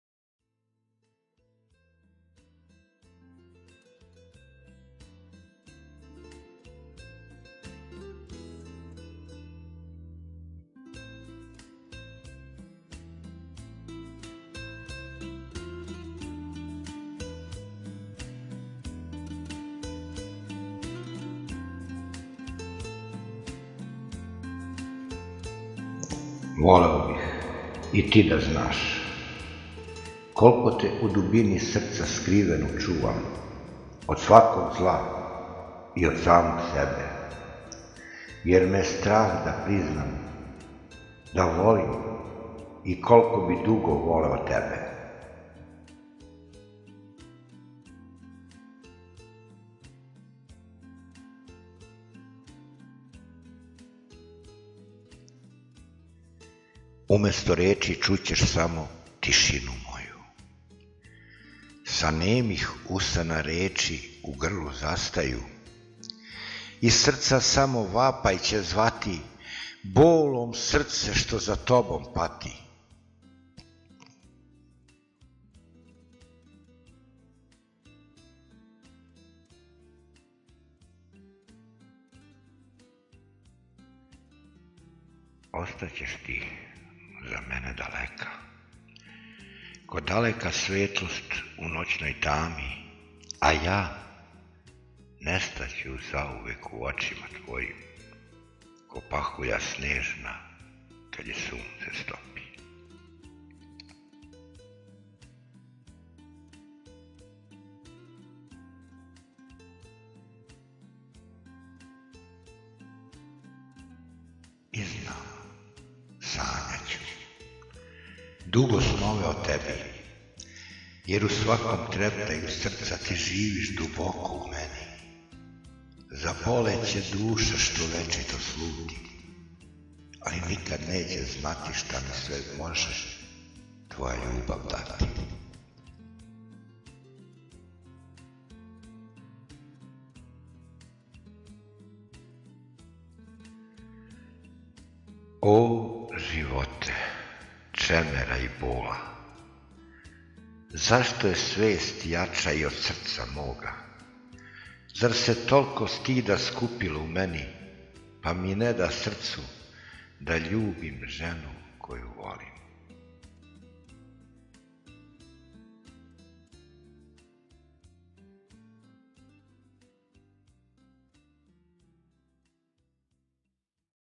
Čuješ li jecaj srca moga....Radovan Bocović
Stihove govori autor pesme Radovan Bocović